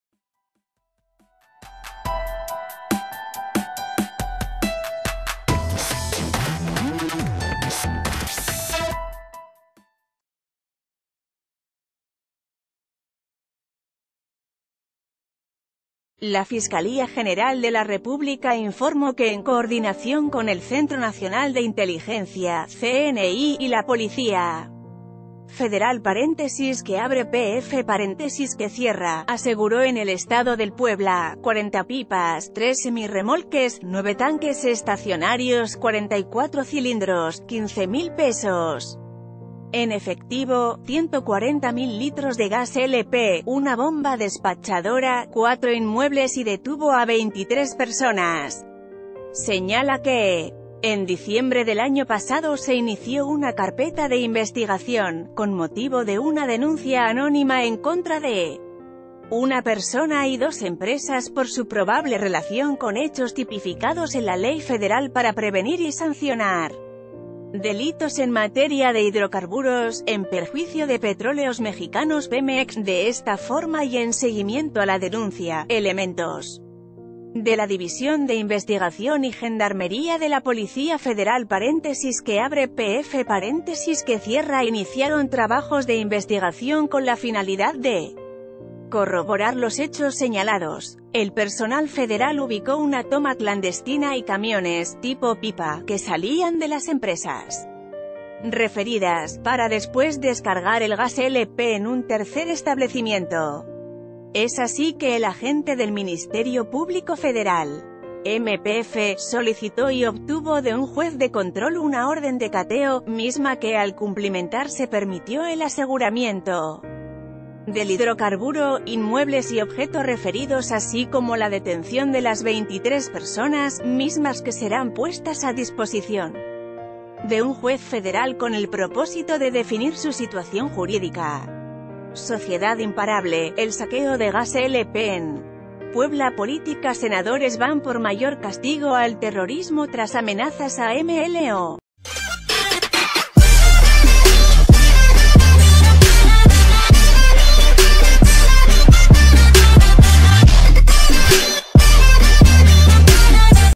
Aseguran 140 mil litros de Gas LP y detienen a 23 personas en Puebla
La Fiscalía General de la República informo que en coordinación con el Centro Nacional de Inteligencia (CNI) y la Policía Federal (PF), aseguró en el estado de Puebla, 40 pipas, tres semirremolques, nueve tanques estacionarios, 44 cilindros, 15 mil pesos en efectivo, 140 mil litros de Gas LP, una bo...